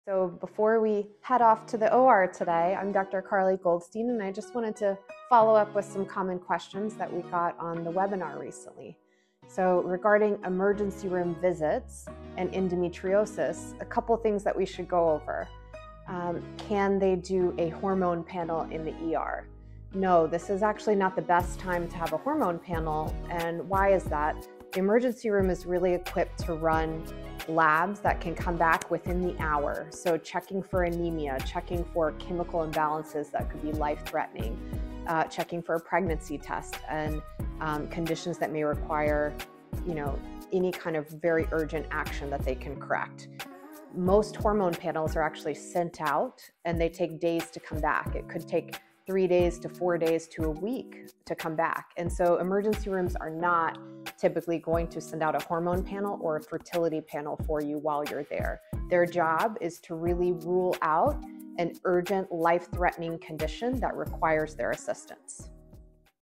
Can You Get a Hormone Panel in the ER? Dr. Goldstein Explains Why Not
In this quick clip, Dr. Karli Goldstein, DO, FACOG—board-certified minimally invasive gynecologic surgeon at ESSE Care—answers a common question: Can the ER run a hormone or fertility panel?

Spoiler: It’s not the best place for that.

Dr. G explains why emergency rooms are designed to handle urgent, life-threatening issues with rapid lab results—think anemia, pregnancy, or serious electrolyte imbalances—not slower, send-out hormone tests that take days to return.

💡 Struggling with hormone imbalance, endometriosis, or fertility concerns? These deserve thorough, integrative evaluation—not a rushed ER visit.

👇 Learn more about our care at ESSE:
📍 Manhattan | Greenwich | Los Angeles